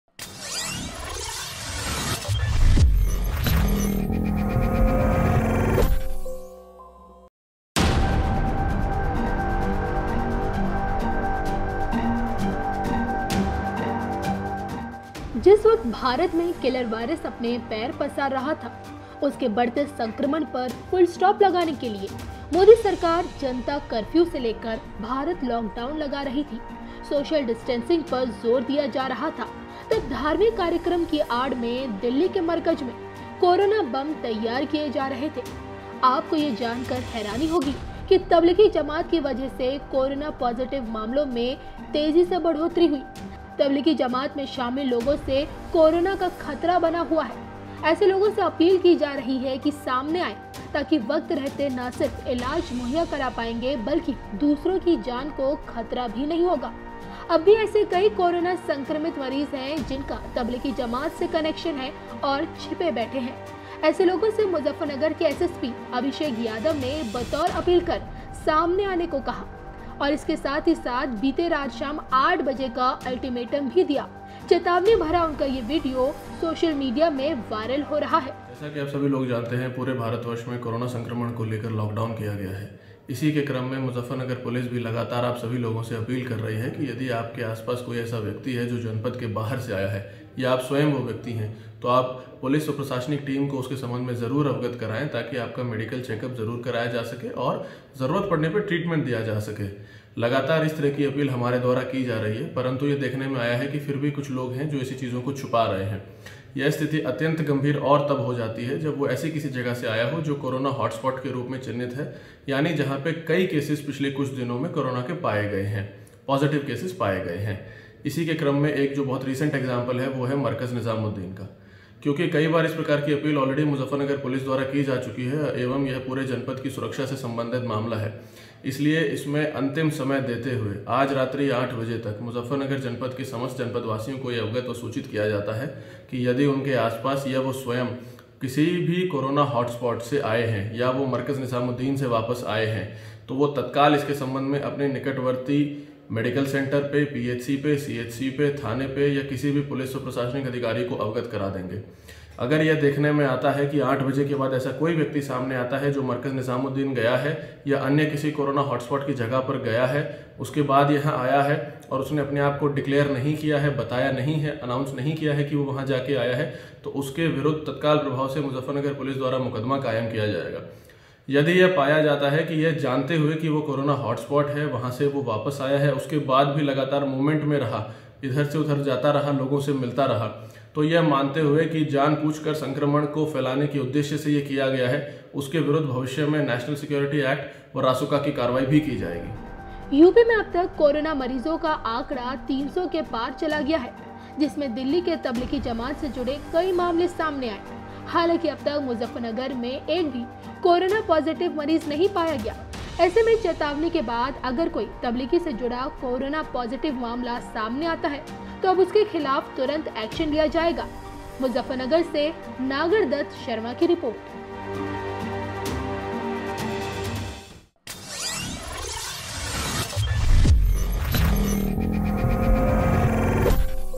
Muzaffarnagar SSP Abhishek Yadav की Jamaat के लोगों को चेतावनी, सामने आओ वरना...
तबलिगि जमात में शामिल लोगों से कोरोना का खतरा बना हुआ है...ऐसे लोगों से अपील की जा रही है कि सामने आए, ताकी वक्त रहते ना सिर्फ इलाज मुहैया करा पाएंगे, बल्कि दूसरों की जान को खतरा भी नहीं होगा….अब भी ऐसे कई कोरोना संक्रमित मरीज हैं, जिनका तबलिगि जमात से कनेक्शन हैं…और छिपे बैठे हैं…ऐसे लोगों से मुज्जफरनगर के एसएसपी अभिषेक यादव ने बतौर अपील कर सामने आने को कहा, बल्कि बीती शाम रात 8 बजे का अल्टीमेटम भी दिया था…..चेतावनी भरा उनका ये वीडियो सोशल मीडिया में वायरल हो रहा है….